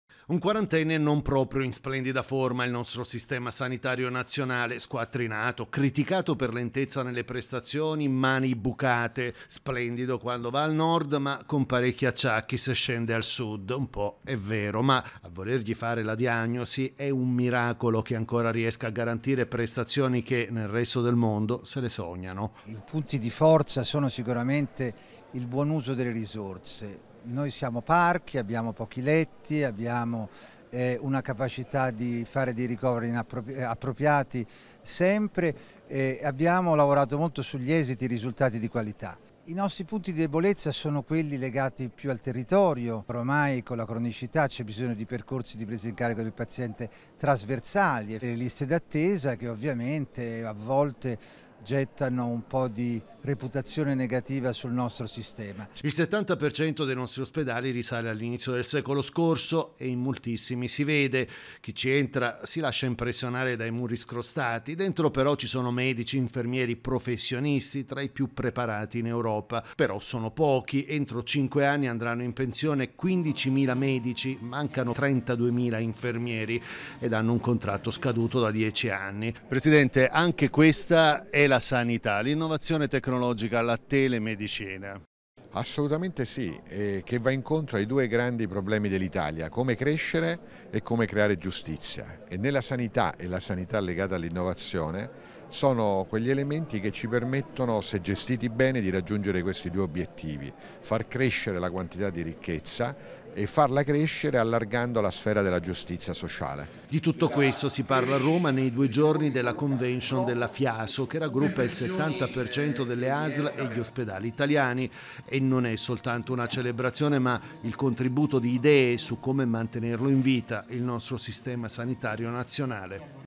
Il servizio di RAI News 24 sulla 1^ Convention del Management della Sanità italiana
Il servizio di RAI News 24 sulla 1^ Convention del Management della Sanità italiana svoltasi a Roma dal 7 al 9 novembre 2018